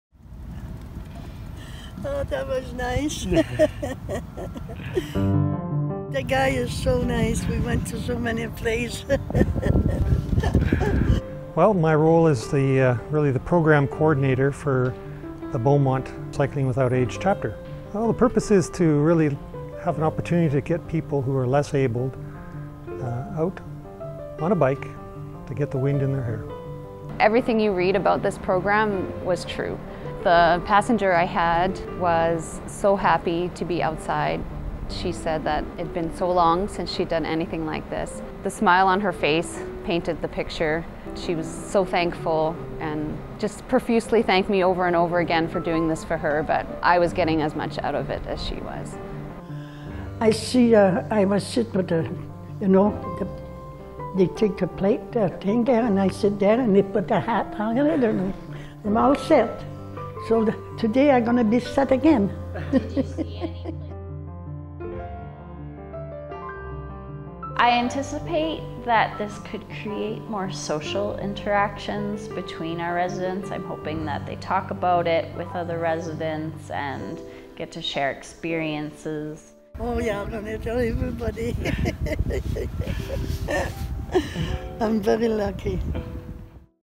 Cycling Without Age
Chateau Vitaline residents enjoy bike rides thanks to a specialized, three-wheeled bicycle called a trishaw.

Cycling Without Age was introduced to Beaumont in August. The international program began in 2012, in Copenhagen, Denmark and has since expanded to 37 countries.